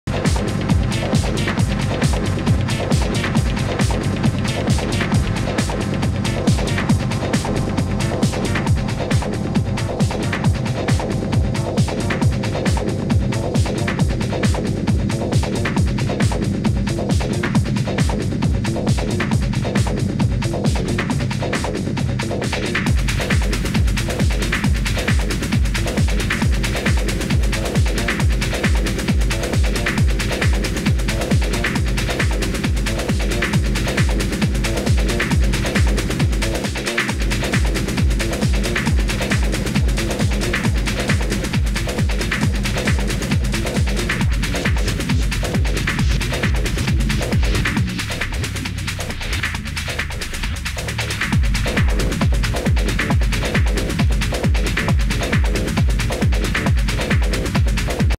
Tom Linder Detroit Techno Militia - techno-club.net Grey Room - Aug 6, 2021
Grey Room Host Detroit Techno Militia Launching sonic missiles Live from Detroit mega city. Tom Linder at the helm..

Free 7 day Trial available w/ recurring subscriptions..